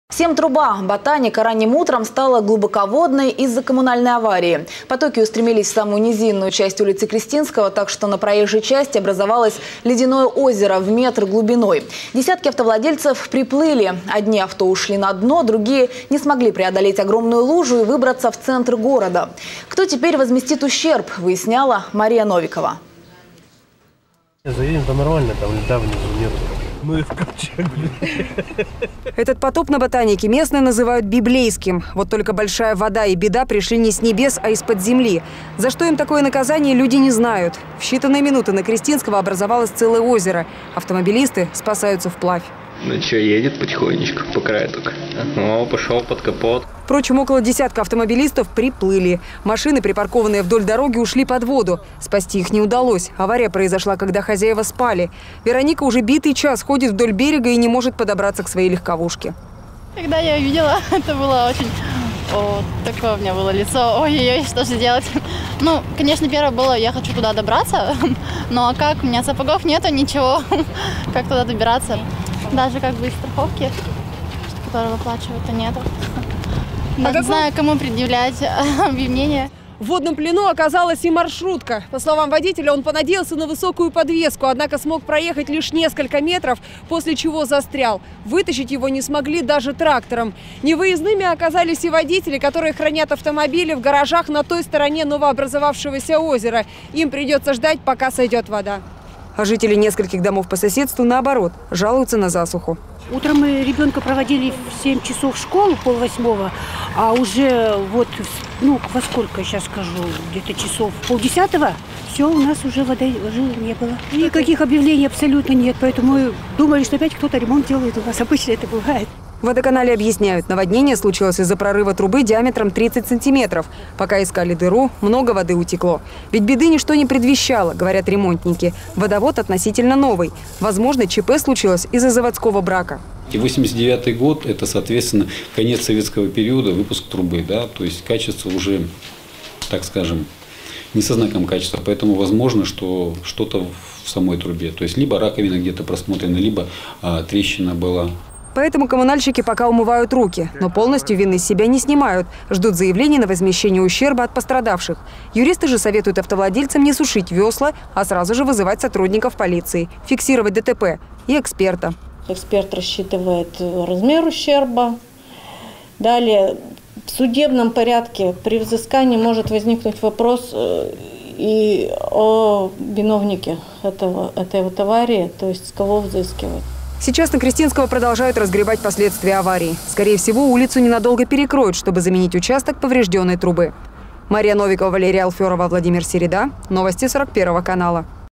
На Ботанике образовалось ледяное озеро
Всем труба:  на Ботанике разлилось ледяное озеро глубиной метр

Приплыли и пешеходы и десятки автовладельцев

Этот потоп на Ботанике местные называют библейским. Вот только большая вода и беда пришли не с небес, а из-под земли. За что им такое наказание, люди не знают, в считанные минуты на Крестинского образовалось целое озеро. Автомобилисты спасаются вплавь. Впрочем, около десятка автовладельцев приплыли. Машины, припаркованные вдоль дороги, ушли под воду. Спасти их не удалось, авария произошла, когда хозяева спали. Вероника уже битый час ходит вдоль берега и не может подобраться к своей легковушке. В водном плену оказалась и маршрутка. Водитель понадеялся, что благодаря высокой посадке он сможет проехать, однако ему удалось преодолеть лишь несколько метров. Транспорт не смогли вытащить даже трактором. Невыездными оказались и автомобилисты, хранившие транспорт в гаражах на другой стороне новообразовавшегося озера. А жители нескольких домов по соседству, наоборот, жалуются на засуху. С утра из кранов пропала вода. В водоканале объясняют, наводнение случилось из-за порыва трубы диаметром 30 сантиметров. Пока искали дыру, много воды утекло. Ведь беды ничего не предвещало, говорят ремонтники, водовод относительно новый. Возможно, ЧП случилось из-за заводского брака в трубе. Поэтому коммунальщики пока умывают руки, но полностью вины с себя не снимают, ждут заявлений на возмещение ущерба от пострадавших. Юристы же советуют автовладельцам не сушить весла, а сразу же вызвать сотрудников полиции и фиксировать ДТП. Сейчас на Крестинского продолжают разгребать последствия аварии. Скорее всего, улицу ненадолго перекроют, чтобы заменить участок поврежденной трубы.